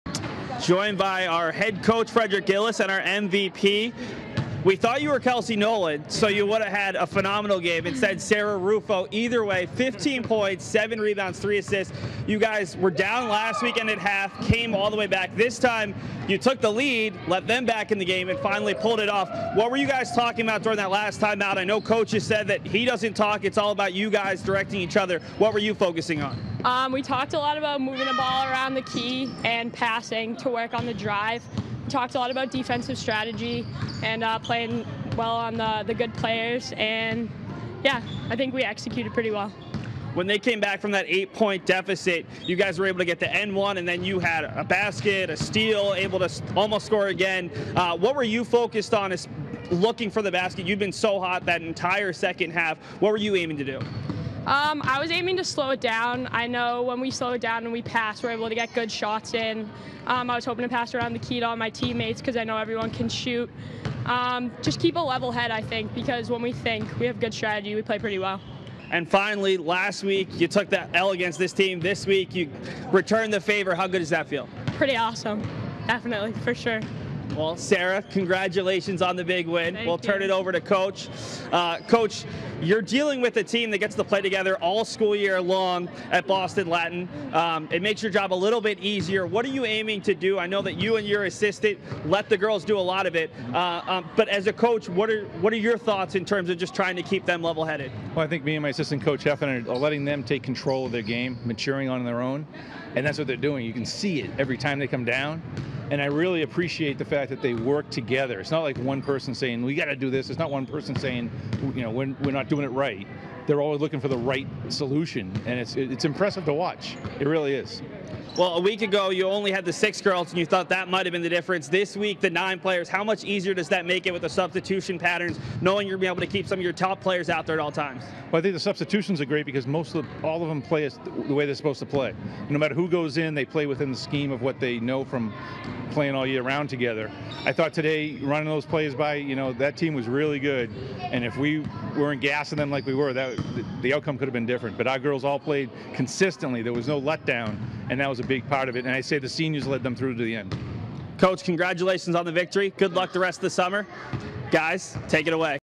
Game of the Week MVP Interview: Wolfpack's Sara Rufo & HC Fredrick Gillis
Game of the Week sideline reporter Seth Orensky interviews game MVP Sara Rufo (15 points, 7 rebounds, 3 assists) and Wolfpack head coach Fredrick Gillis, after their team defeated the Ravens, 48-45, at the Tynan Community Center in South Boston, in the Girls 18 & Under Division of the BNBL.  To view the Ravens vs. Wolfpack Girls 18 & Under BNBL basketball game in its entirety, please